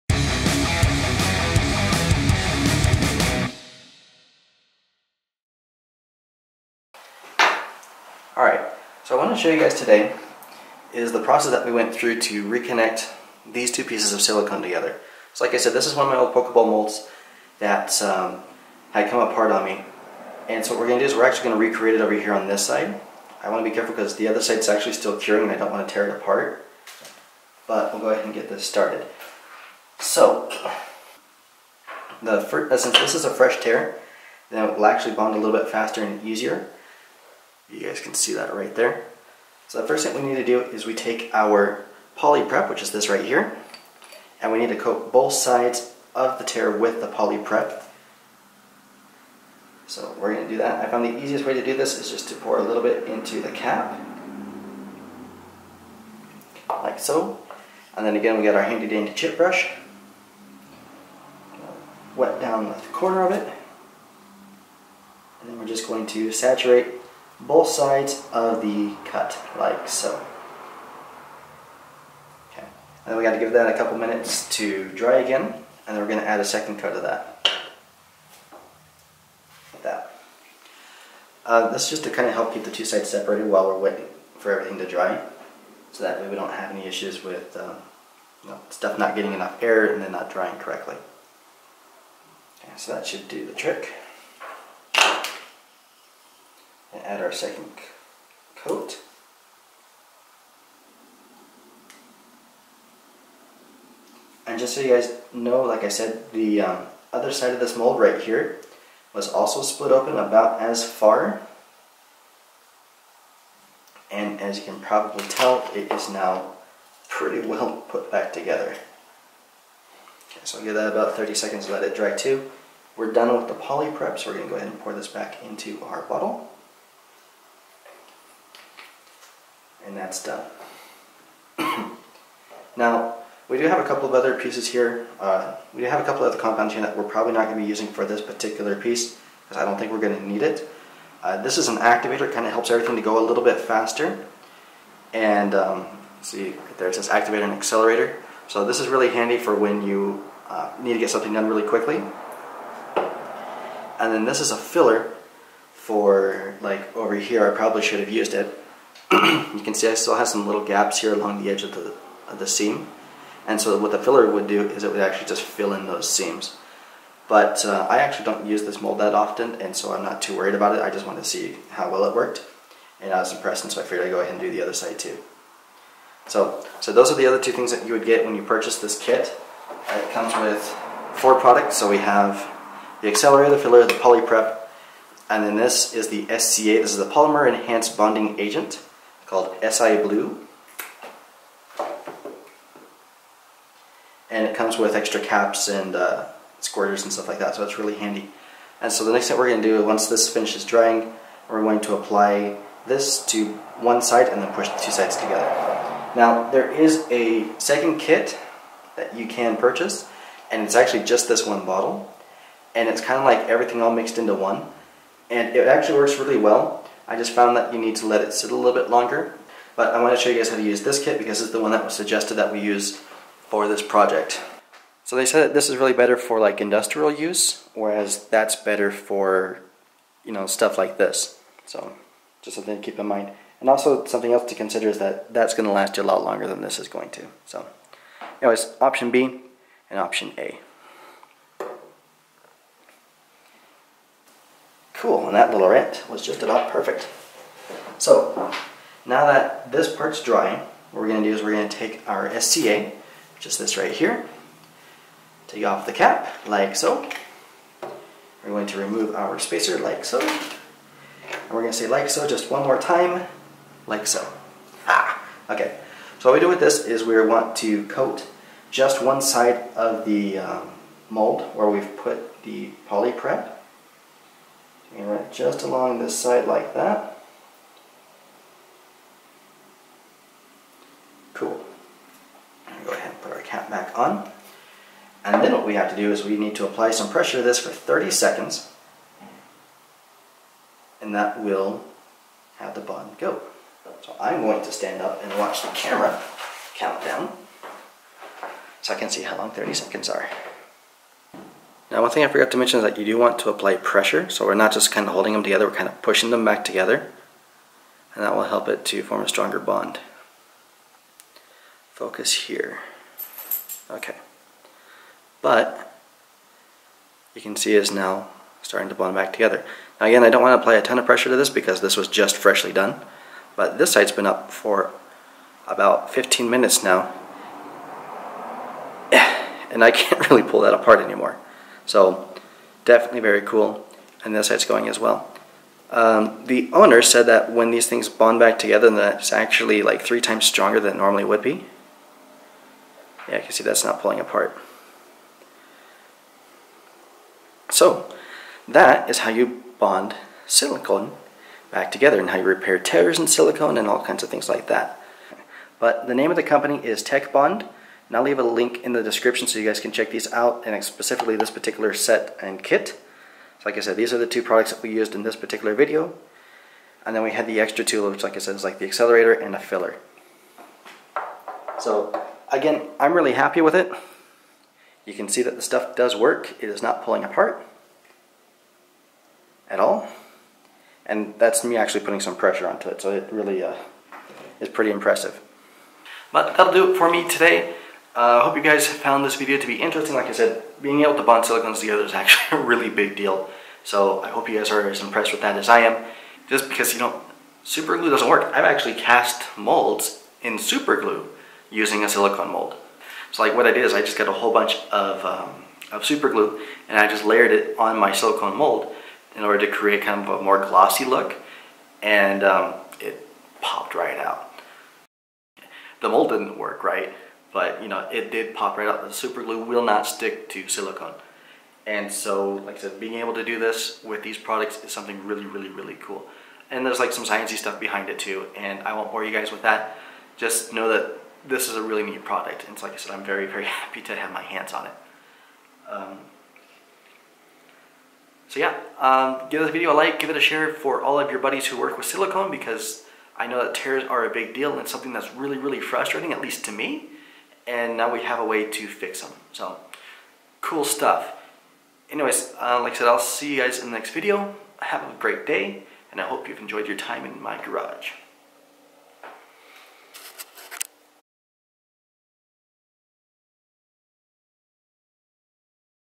How to Repair Torn Silicone - Best glue for Silicone - Tech-Bond Review
Our Newest Video: "Automatic Pokeball Factory
I got a neat present today from the folks at Tech-Bond - an adhesive that will glue silicone pieces together!

So, you're probably thinking that a silicone glue isn't really that big of a deal, but let me ask you a question: have you ever had a silicone mold tear? Have you ever tried to put it back together? It's not easy... That's why this adhesive is so impressive. Not only was I able to repair my mold in about 2 minutes, but that thing is not coming apart now. I've tried using other adhesives and glues to fix that thing, it was one of my favorite molds, but nothing worked - not even super glue or Gorilla Glue or any of the other glues on the market. Not only did this one work, but I recorded the whole thing, the entire process so that you can see it for yourself. If you have any torn silicone molds, I'd definitely suggest that you take a look at Tech-Bond's line of products. They're awesome.